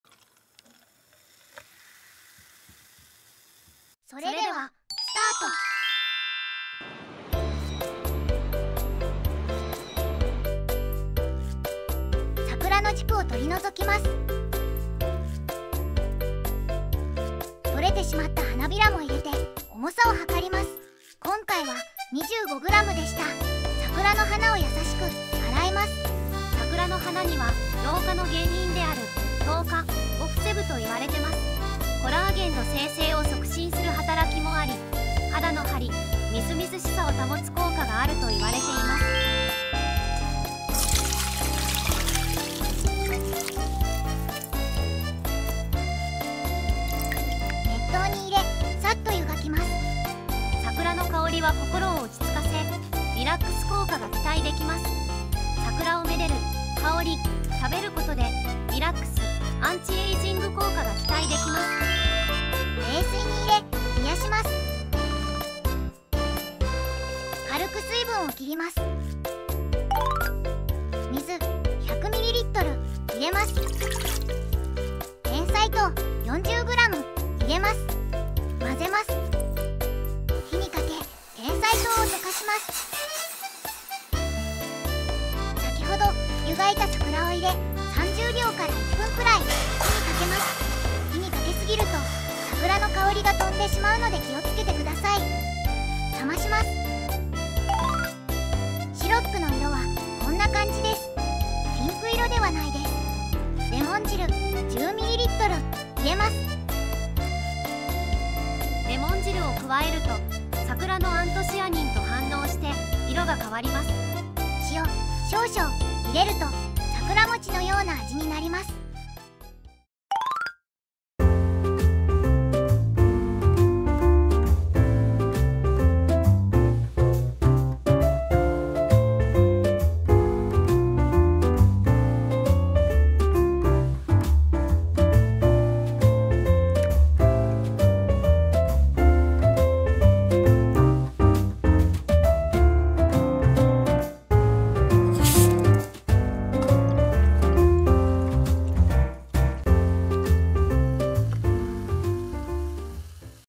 【春の香りでリラクッス♪】期間限定🌸桜シロップの作り方 薬膳
春らしくなり、桜の季節です🌸
桜の花は老化の原因である「糖化」を防ぐと言われています

桜を愛でる、香り、食べることで、リラックス、アンチエイジング効果が期待できます

【再生リスト】

【材料】
桜の花（今回は河津桜を使用）　25ｇ
水　100ml
てんさい糖　40ｇ
レモン汁　10ml
塩　少々

1.桜の花から軸を取ります。取れてしまった花びらも使ってください
2.重さを量ります
3.桜の花をやさしく洗います
4.水分を切り、熱湯に入れ、さっと湯がきます（苦みを取るため）
5.冷水に入れ、冷やし、水分を軽く切ります
6.鍋に水、てんさい糖を入れて混ぜます
7.火にかけ、てんさい糖を溶かします
8.7に5を入れ、30秒－１分くらい火にかけ、冷まします
9.冷めたら、レモン汁、塩を入れて混ぜます
10.１－２週間くらいおくと、苦みも弱まり、きれいな色になります
豆乳プリンにかけたり、お湯、炭酸で割って飲んでください

【クックパッド】にゃんたろう薬膳 のキッチン

薬膳 レシピ
薬膳 簡単